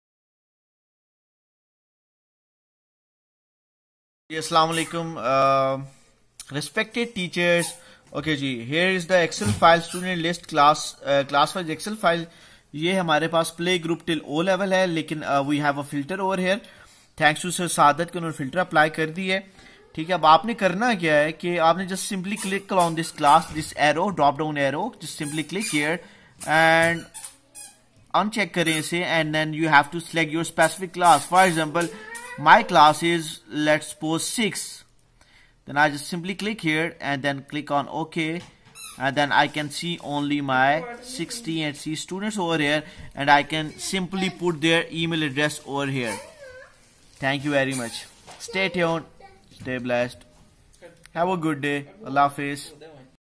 How to Apply Filter Using Ms Excel Sheet
This tutorial will help you that how to filter data /or how to extract specific data by applying filters